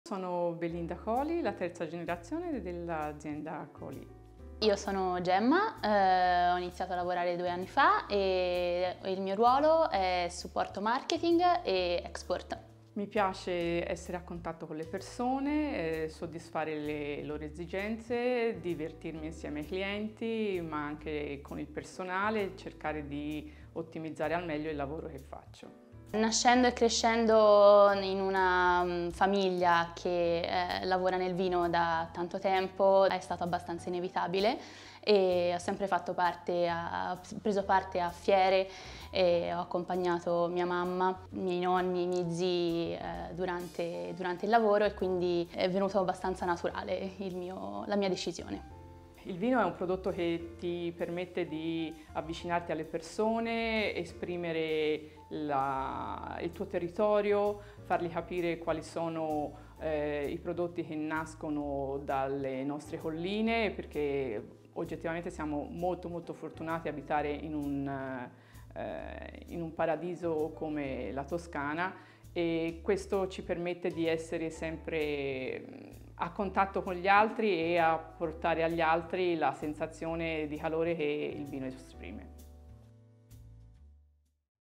Coli
“Mi piace essere a contatto con le persone, soddisfare le loro esigenze e divertirmi insieme ai clienti. Il vino ti permette di avvicinarti alle persone, esprimere il tuo territorio e far capire quali sono i prodotti che nascono dalle nostre colline. Siamo molto fortunati ad abitare in un paradiso come la Toscana.”

“I like meeting people, helping them and I enjoy being with clients. Wine is a product which allows you to become closer  people, to explain the land and help them understand the products which our hills provide. we are so so lucky to live in a paradise like Tuscany.”

Azienda / Estate:  Coli
Intervistato / Interviewee: Belinda Coli e Gemma Caiani